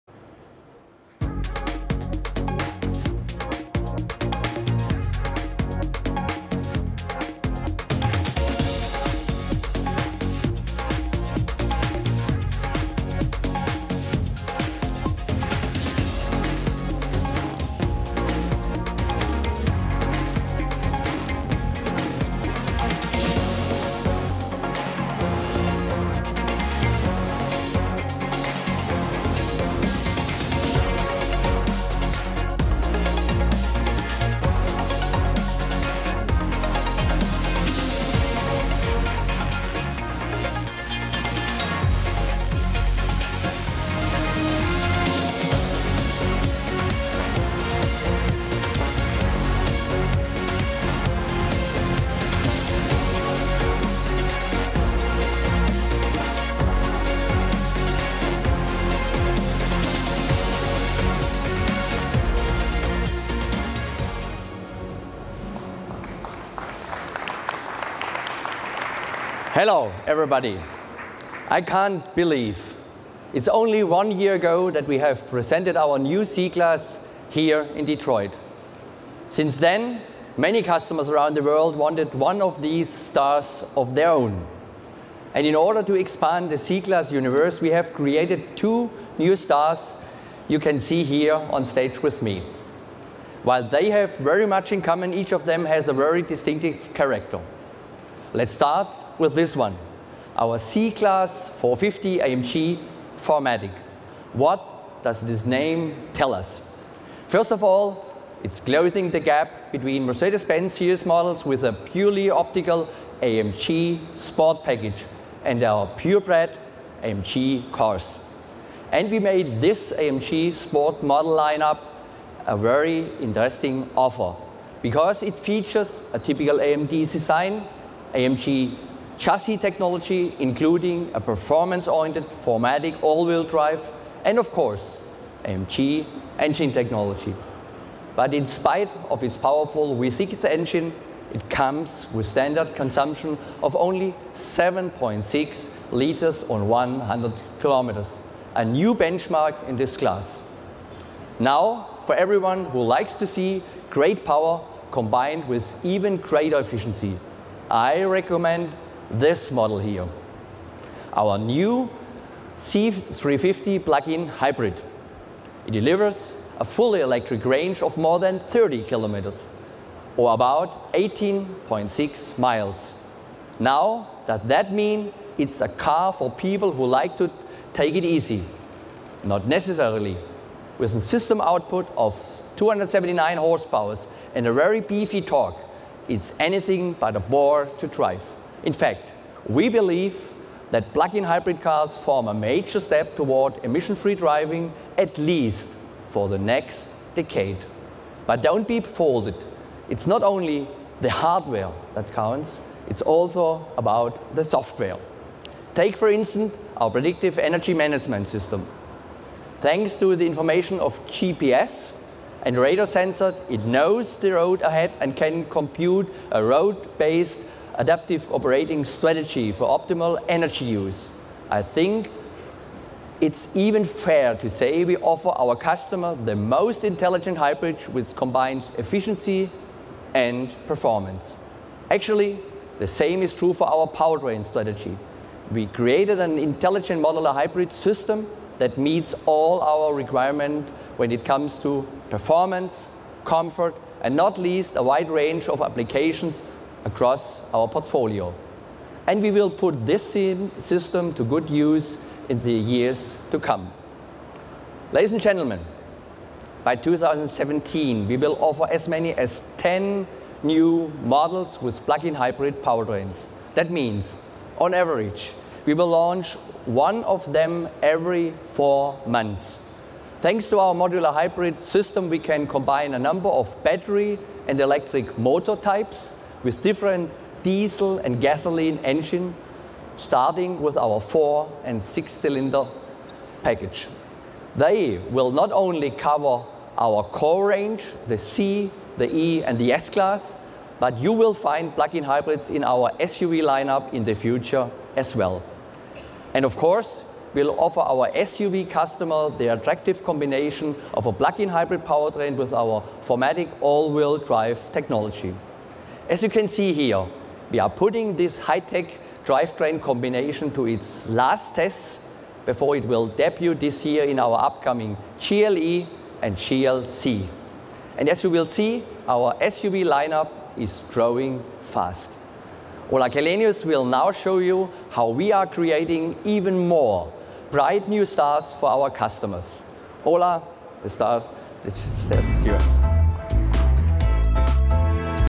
NAIAS 2015 - Mercedes-Benz Press Conference
Speech Prof. Dr. Thomas Weber - Member of the Board of Management of Daimler AG, responsible for Group Research and Mercedes-Benz Cars Development